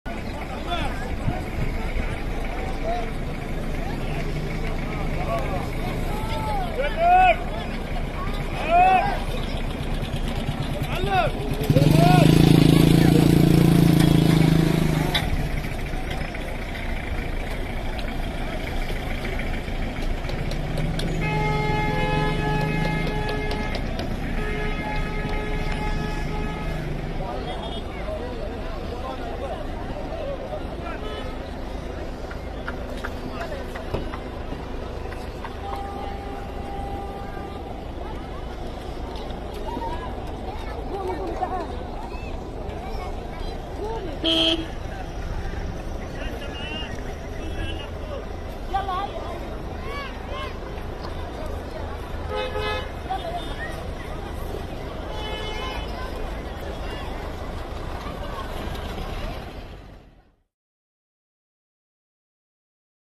Palestinians Flee To Egyptian Borders
Amid heavy bombardments across the entire Gaza Strip, thousands of internally displaced Palestinians are moving south towards the Egyptian border.